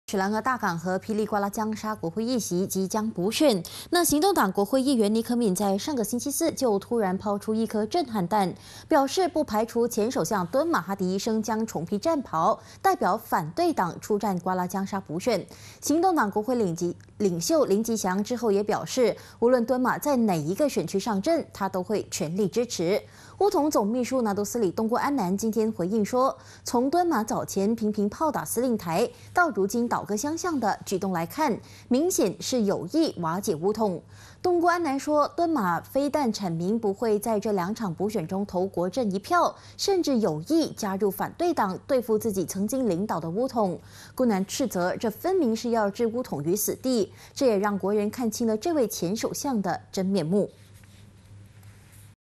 倒戈对付国阵 姑南:敦马欲置巫统于死地 [20160516]
雪兰莪大港和霹雳瓜拉江沙国会议席，即将在来临的6月18号进行补选。行动党国会议员倪可敏上个星期四突然抛出一颗震撼弹，表示，不排除前首相敦马将重披战袍，代表反对党出战瓜拉江沙补选。行动党国会领袖林吉祥，之后也表示，无论敦马在哪一个选区上阵，他都全力支持。